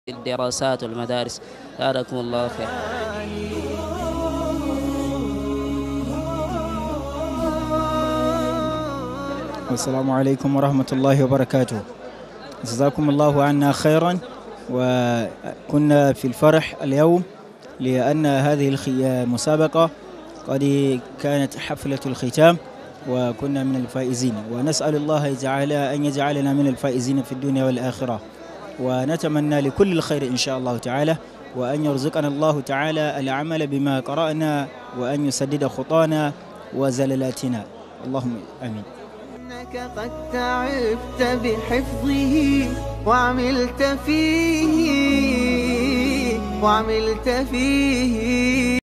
2018 Saudi Arabia King Abdul'aziz Musabaqah | AbdulGaniyy Aminu | Nigeria | 60 Hizb 3rd | Farewell
2018 Saudi Arabia King Abdulaziz Musabaqa | AbdulGaniyyu Aminu | Nigeria | 60 Hizb 3rd | Parting Words